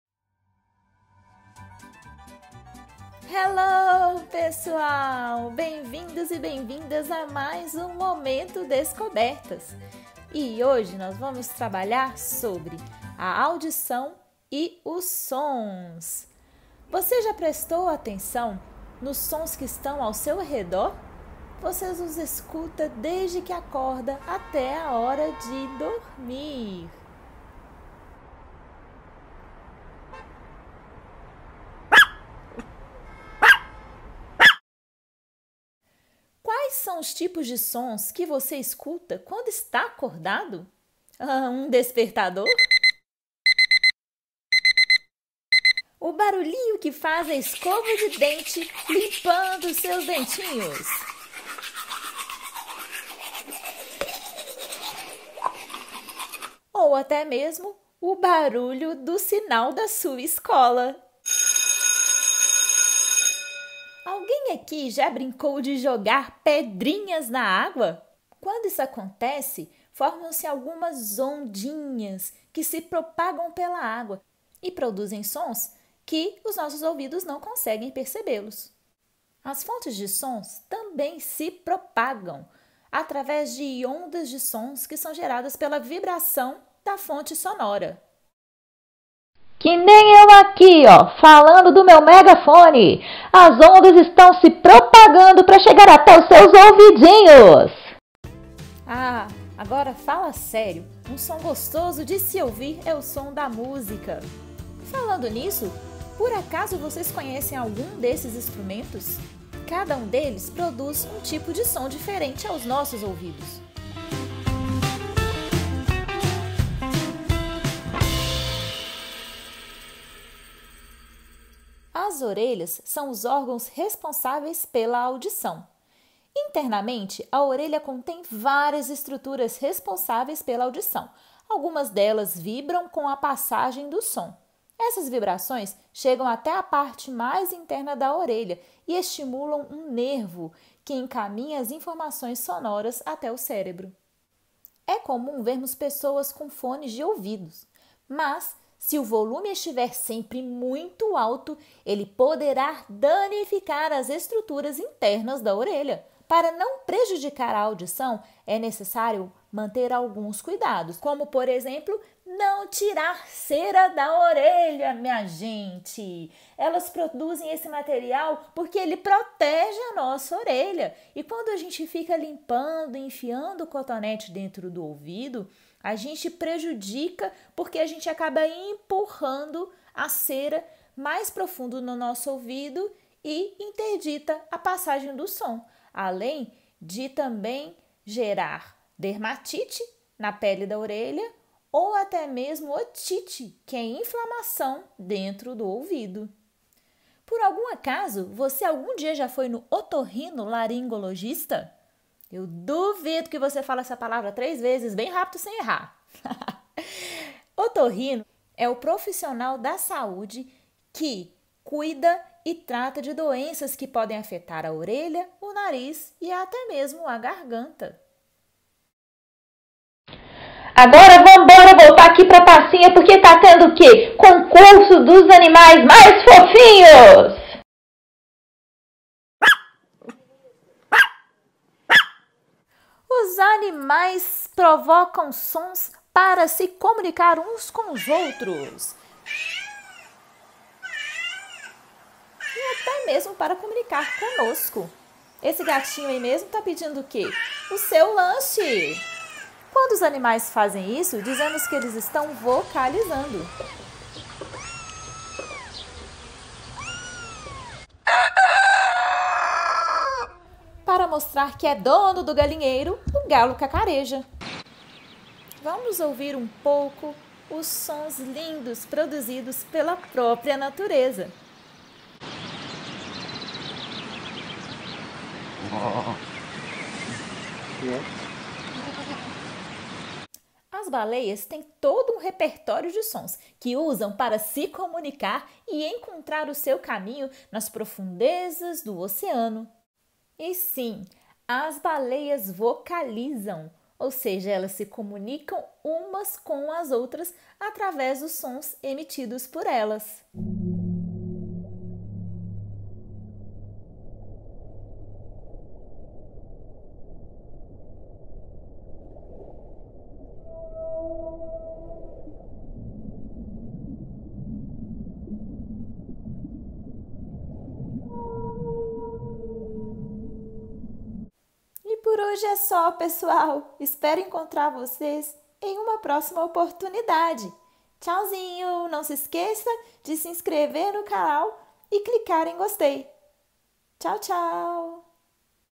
A audição e os sons ao nosso redor
Como ouvimos?

Olá, amigos e amigas! Segue um vídeo super legal e cheio de curiosidades, como vocês gostam!

(EF03CI01) Produzir diferentes sons a partir da vibração de variados objetos e identificar
variáveis que influem nesse fenômeno.
(EF03CI03) Discutir hábitos necessários para a manutenção da saúde auditiva e visual considerando as condições do ambiente em termos de som e luz.

Conteúdo inspirado no livro da Editora FTD e SAS.